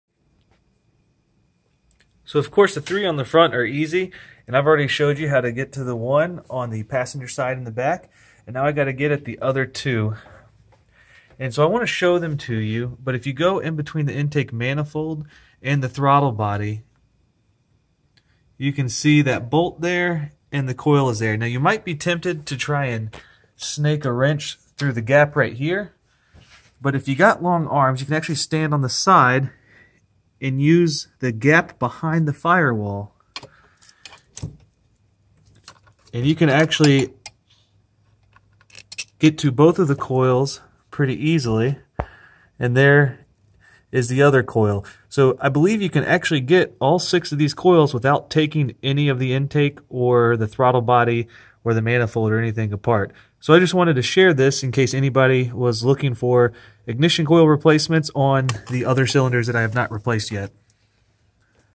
1999 Lexus ES300 - Ignition Coils 3 and 5
Quick tips on replacing ignition coils 3 and 5 in a Lexus ES300. These are on the rear bank under the intake manifold. There is just enough room to swap them out without removing the throttle body or intake manifold.